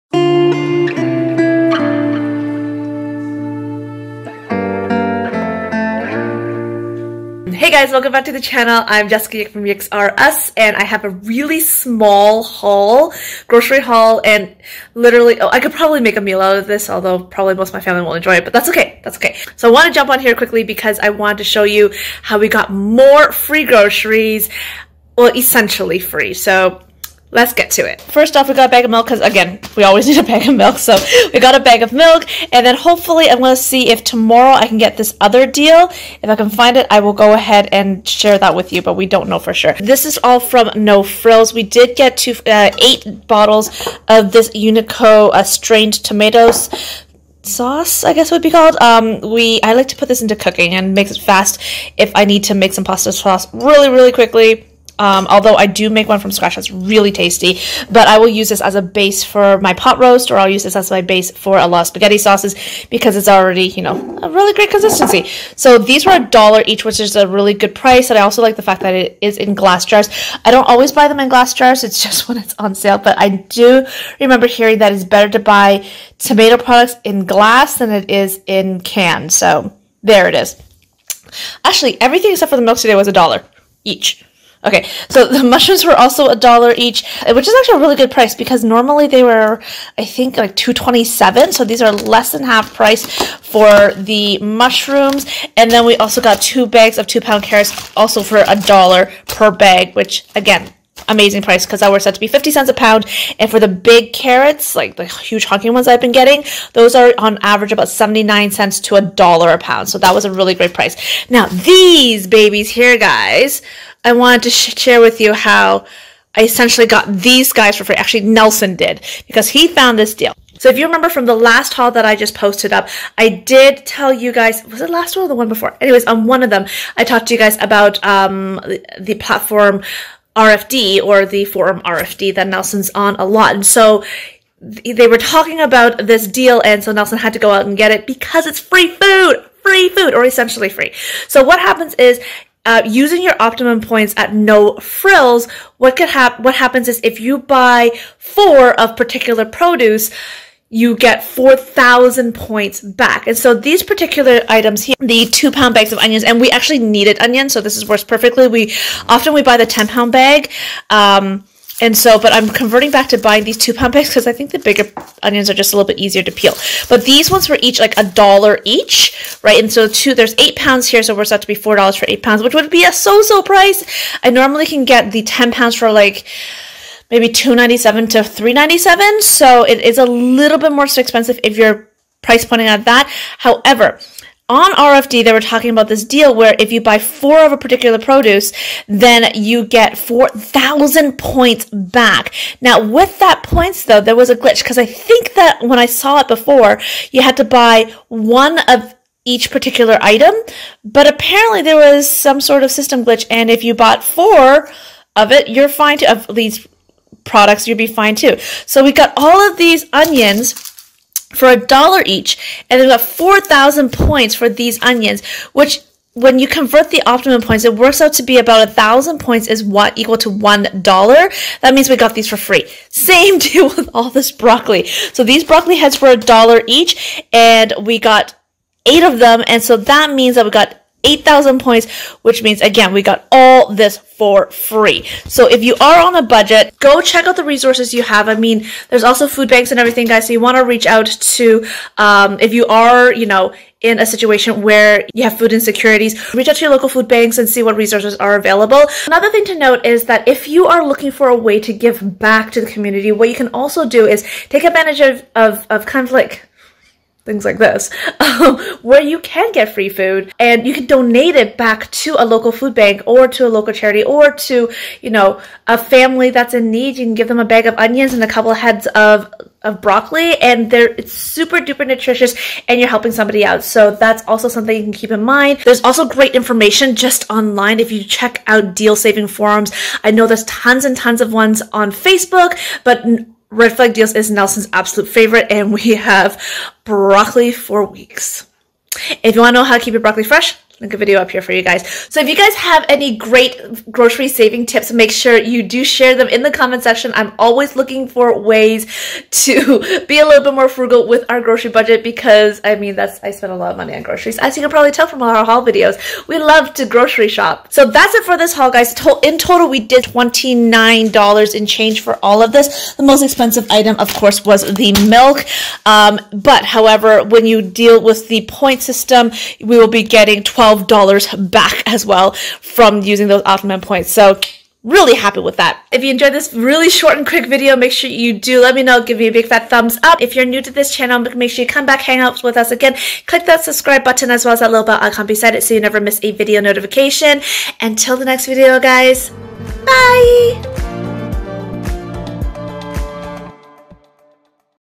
How we got some FREE Groceries! | Grocery Haul | YiksRUs
Hi friends!  I'm Jessica and welcome to YiksRUs.  Today I'm sharing a small haul but in this haul, we were able to get essentially 4 bags of onions and 8 heads of broccoli for free!

If you are experiencing or know of someone experiencing food insecurities, please see if you can get help from your local resources such as

Get $5 Cash Back from online shopping when you sign up for a new account

Much love!

YiksRUs

All opinions expressed are our own